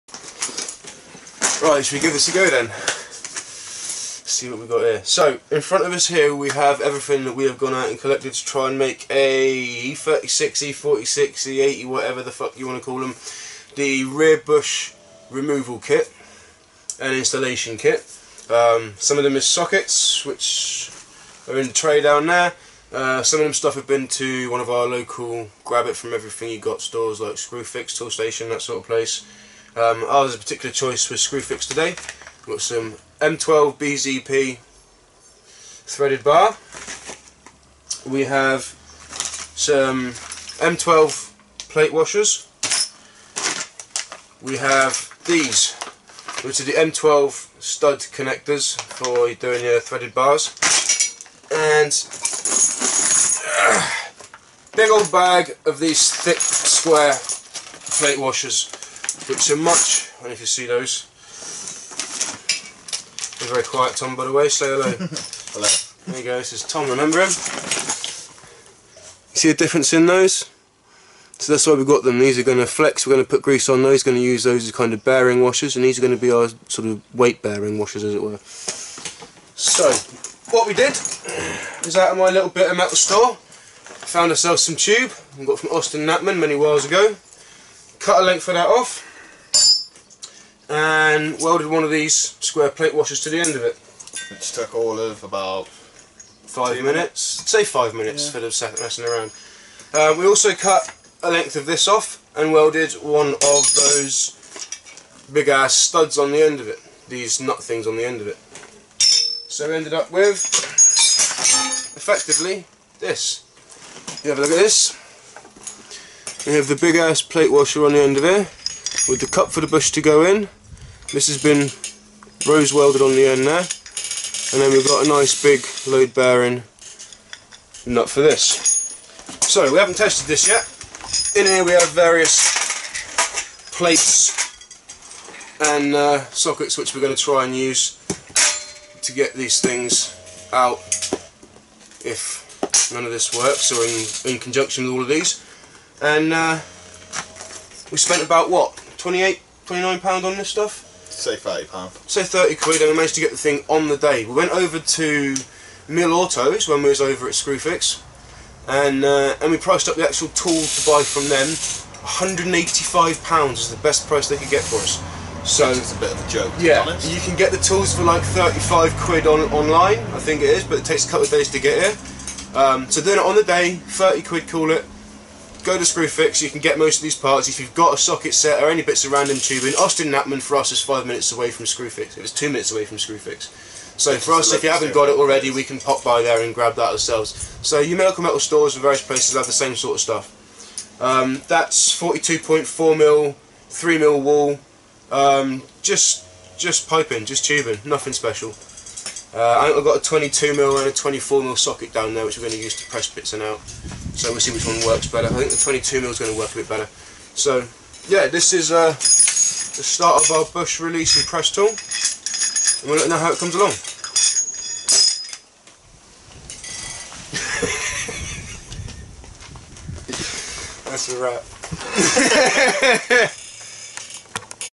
The John & Tom Show ep1pt2 BMW E36 Home Made Rear Control Arm Bush Tool. @ John's Junkyard
In this segment we give you a quick run down of the kit we use to make our BMW E36 rear control arm bush tool, Find out whether it works and any modifications we make in the next episode.